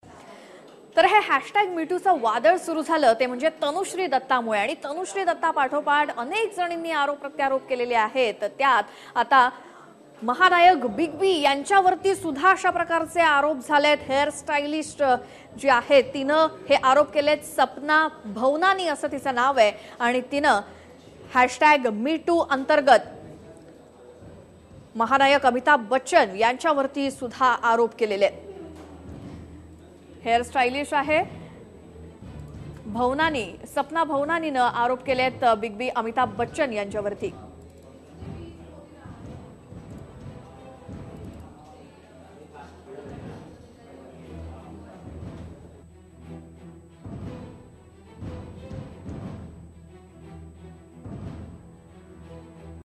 Amitabh Bachchan named in MeToo | Celebrity stylist Sapna Bhavnani Tweet on Him-TV9
celebrity hairstylist Sapna Bhavnani took to Twitter to attack veteran actor Amitabh Bachchan